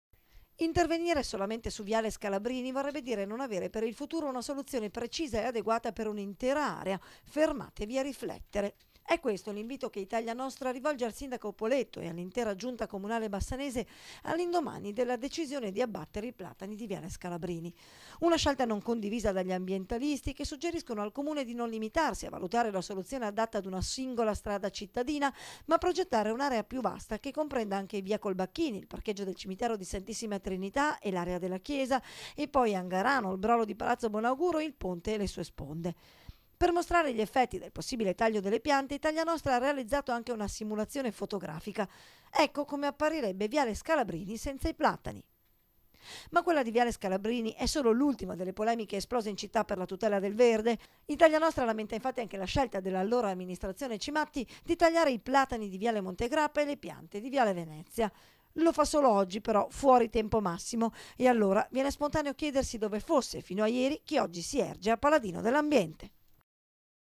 20/02/2015 - VIALE SCALABRINI, PAROLA AGLI AMBIENTALISTI
BASSANO TG (venerdì 20 febbraio 2015) - Torniamo ad occuparci anche oggi di viale Scalabrini e dell’annunciato taglio degli storici platani che lo costeggiano. Sulla questione interviene oggi anche Italia Nostra